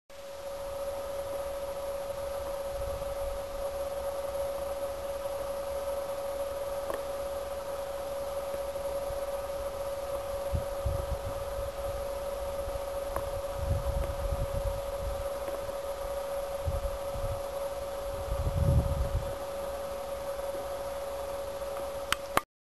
Lacuri dintre Sebes si Daia Romana
Pe drumul spre Daia Romana. 11 Noiembrie 2008.